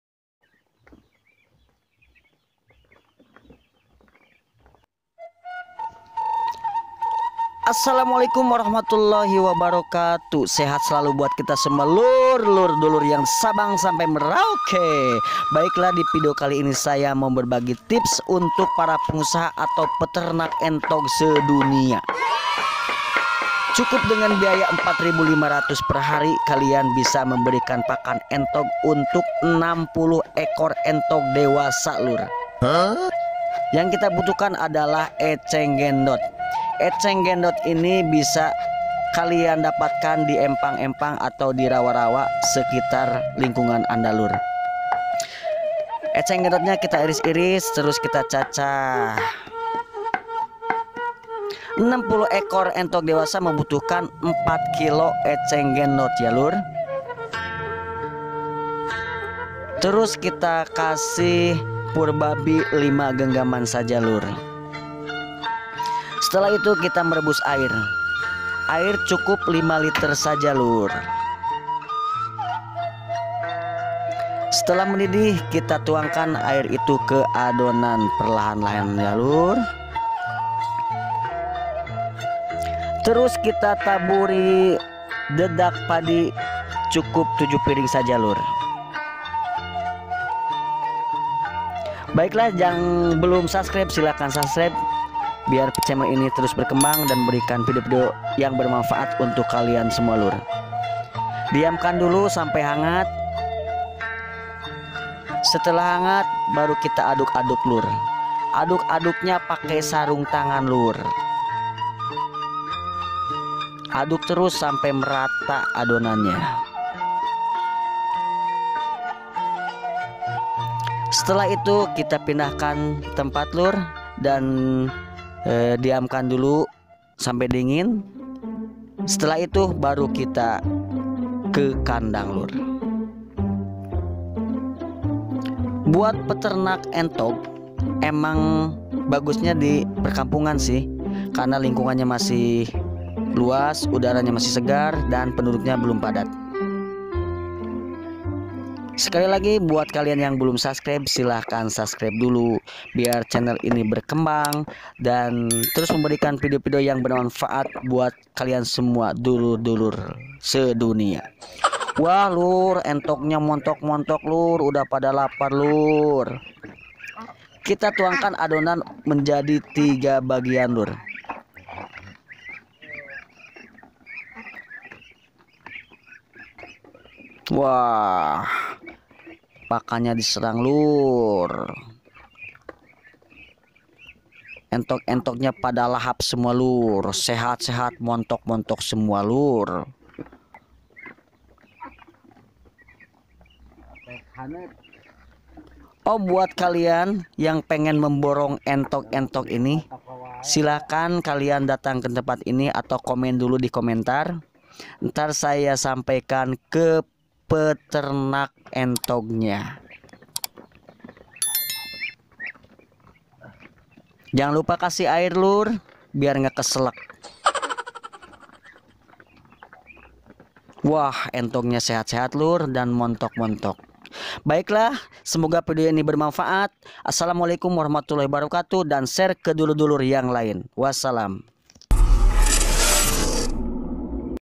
Cukup biaya Rp.4.500 / Hari untuk pakan 60 ekor entok dewasa
Video ini berbagi tips untuk pengusaha atau peternak entok .
Biaya pakan untuk 60 entok dewasa cukup biaya Rp. 4.500 / Hari.
Apa itu pakannya. Kita lihat videonya yu Lur.
Selamat menonton Wassalamu'alaikum